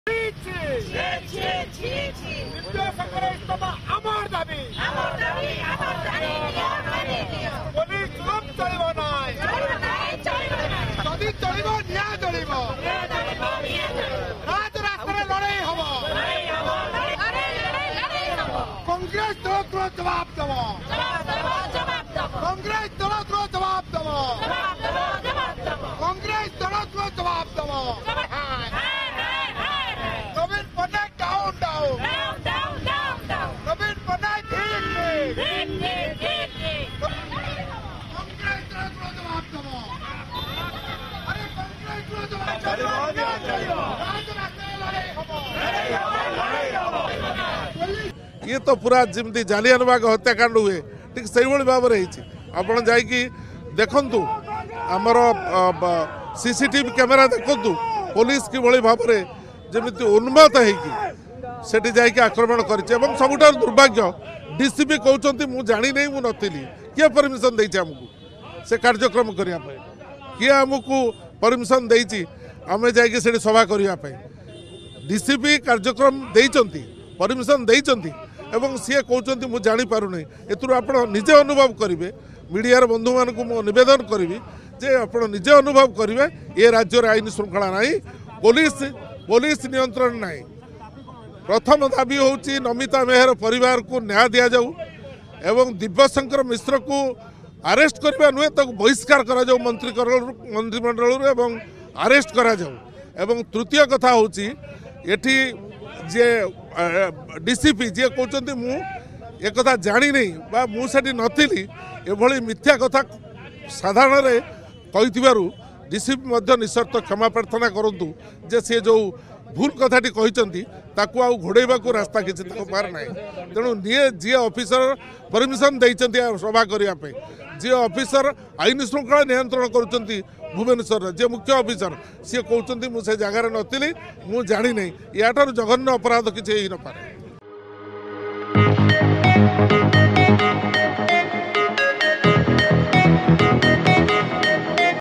‘କଂଗ୍ରେସ କର୍ମୀଙ୍କୁ ଆକ୍ରମଣକରି ବହୁତ୍ ବଡ଼ ଭୁଲ୍ କଲ’ | Odisha Reporter
ଲୋୟର ପିଲମଜିରେ ଦୁଇଦିନ ତଳେ କଂଗ୍ରେସର ଯୁବକର୍ମୀମାନଙ୍କୁ ଯେଉଁଭଳି ଭାବରେ ପୁଲସ ଗୋଡ଼ାଇ ଗୋଡ଼ାଇ ମାଡ଼ ମାରିଥିଲା, ତାକୁ ନେଇ ବରିଷ୍ଠ କଂଗ୍ରେସ କର୍ମୀ ଓ ନେତା ମାନେ ଆଜି ଭୁବନେଶ୍ବର ଡିସିପି ଅଫିସ ଆଗରେ ବିକ୍ଷୋଭ କରିଛନ୍ତି । ପୁଲିସର ଆକ୍ରମଣକୁ ଜାଲିଆନାଓ୍ବାଲାବାଗ ହତ୍ୟାକାଣ୍ଡ ସହିତ ତୁଳନା କରିଛନ୍ତି କଂଗ୍ରେସ କର୍ମୀ ।